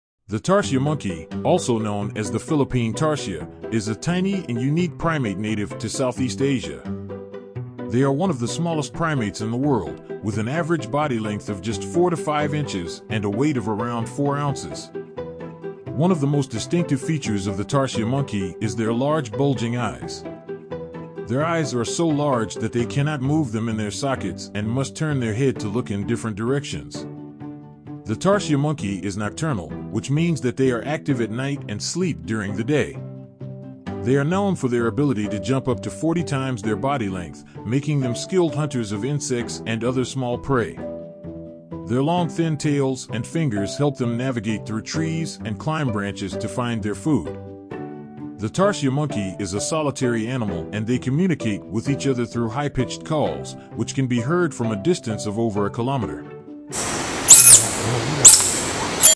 Tarsier Monkey: A Nighttime Marvel.
The Tarsier Monkey, also known as the Philippine Tarsier, is a tiny and unique primate native to Southeast Asia.